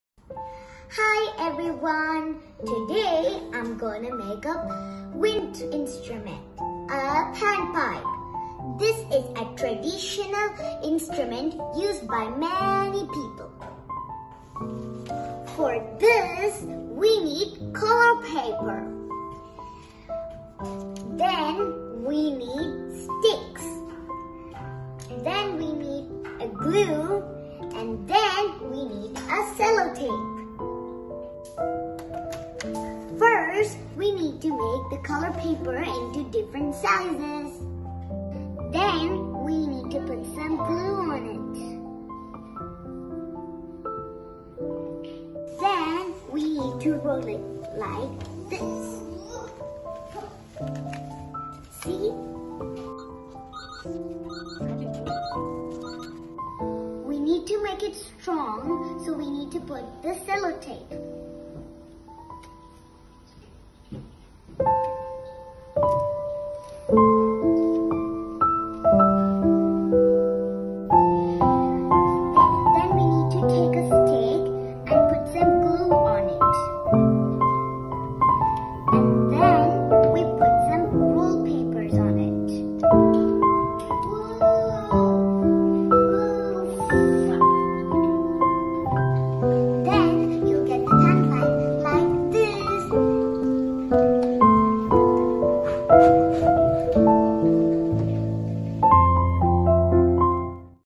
Kid's Project - How to make a musical wind instrument - A Pan Pipe
An easy to make musical wind instrument for school projects (just a replica and not a working one). This is known as a Pan Pipe.